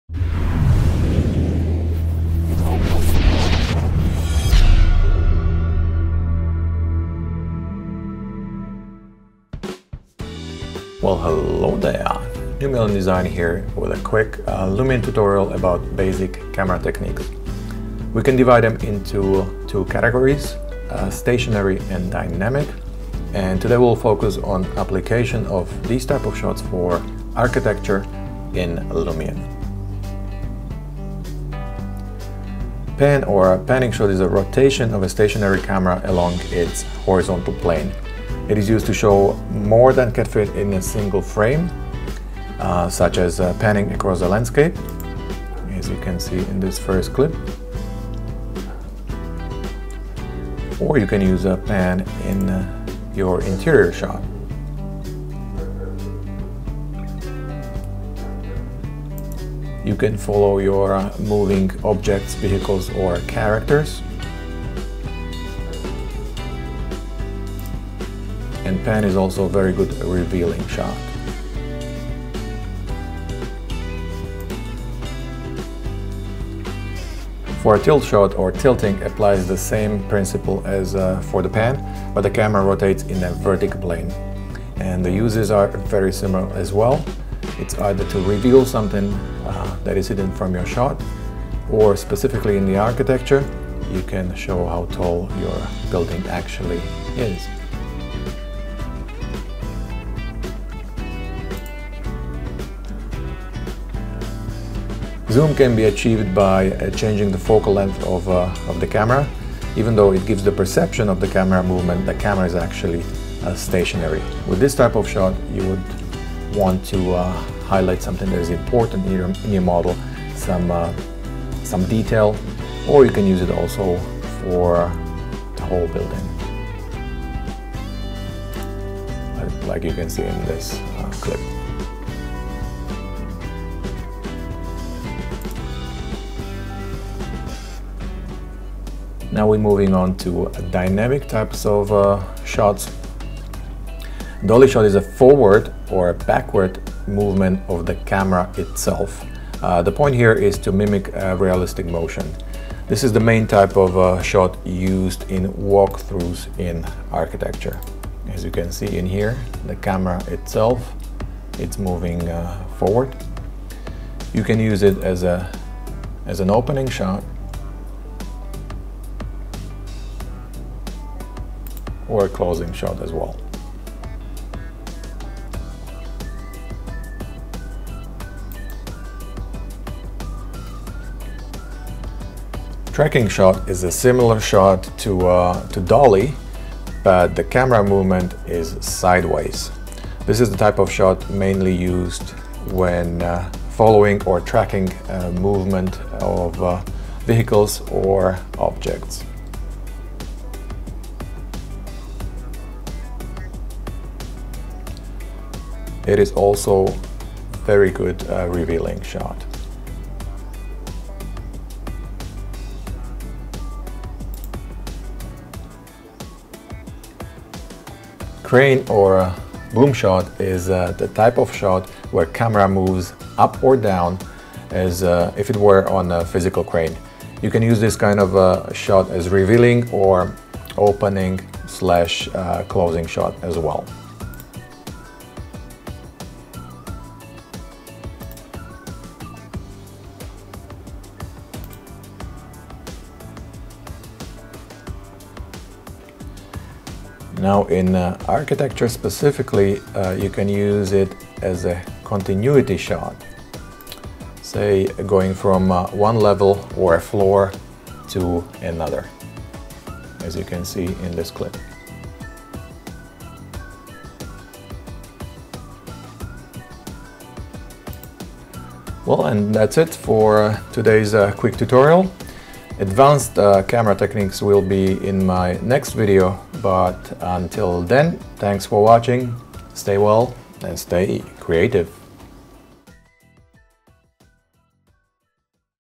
LUMION 8 Basic Camera Techniques Tutorial
Quick Basic Camera Techniques Tutorial for Lumion 8. Types of shots used in Architecture.